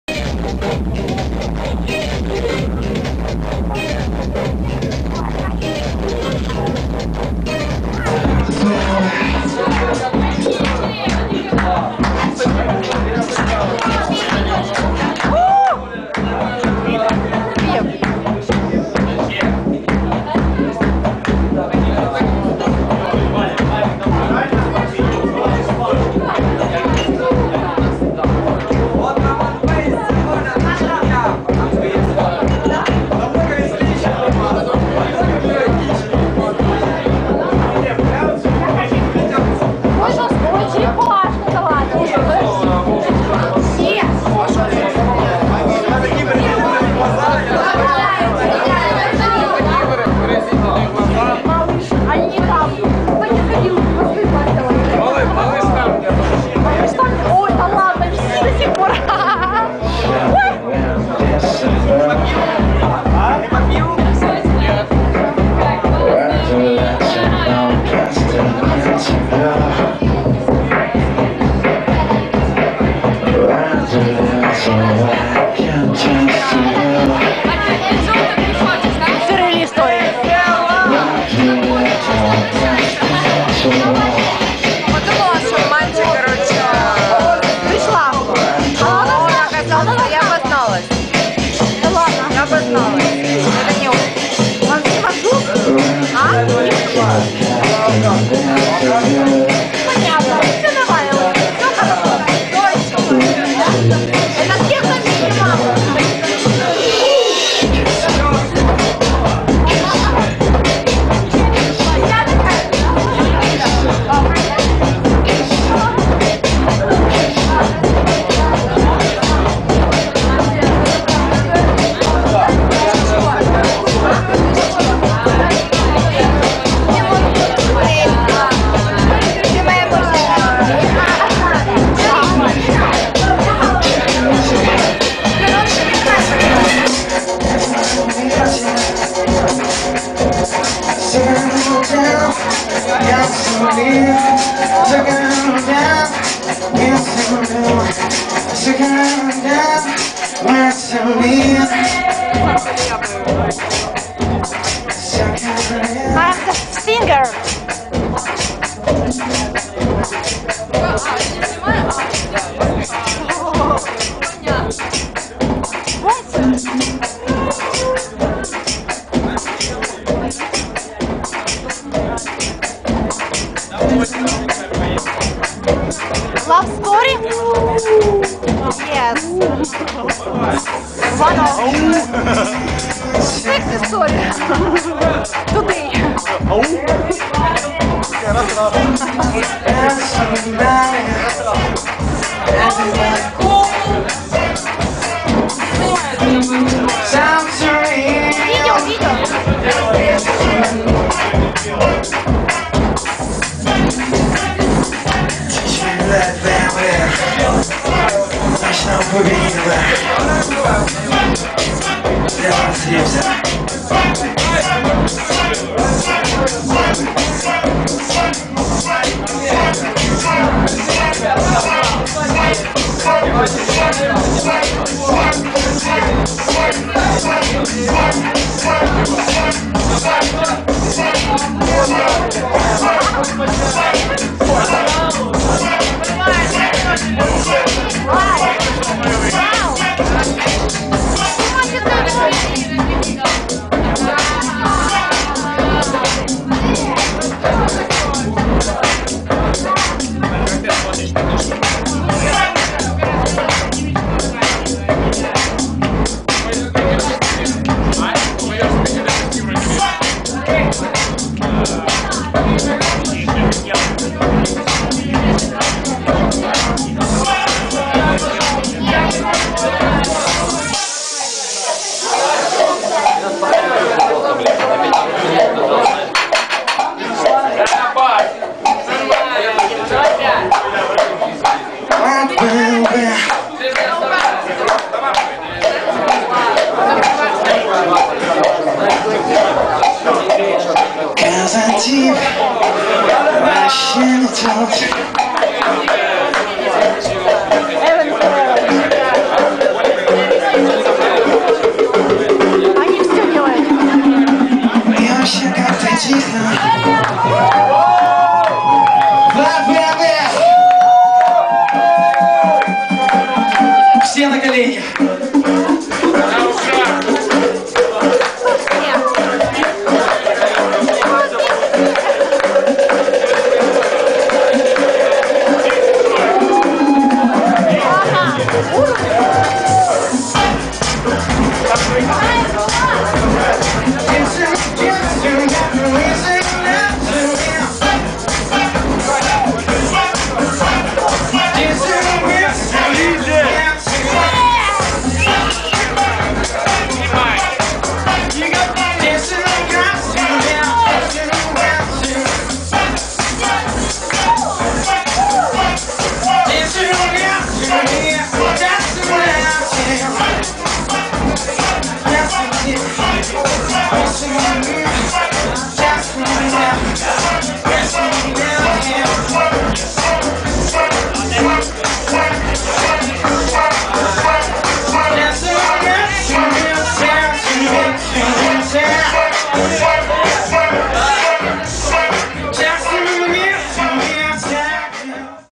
КАZАНТИПВАЩЕНЕТОТ-2 @ Небо 2009 12 12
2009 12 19
Party Bar "Небо"
КАZАНТИПВАЩЕНЕТОТ-2
VladbmV
Dnipropetrovsk
Ukraine

[Не ржач. ЭксклюзивЭ]
Явно не хватало Ricardo Villalobos*а и ZIP (Perlon*а) 1ый коммент от girlfriend]. Днепропетровские диджеи так и не смогли их заменить, хотя в некоторые моменты количество людей, продвигающих "хауз культуру (какого джона?)" за пультом лишь немного уступало количеству, находившихся на танцполе (не хватало шлемов и партизанской формы). Но ребятам не стоит огорчаться за свой "сдвиг на 15 минут", т.к., уверен, их поклонники эти песни сразу прослушали в эфире радио шарманка после действа.
Как сказал один наш земляк: "О какой диджейской этике можно говорить"...

П.С. С удовольствием отмечу, что приятных моментов и людей было значительно больше.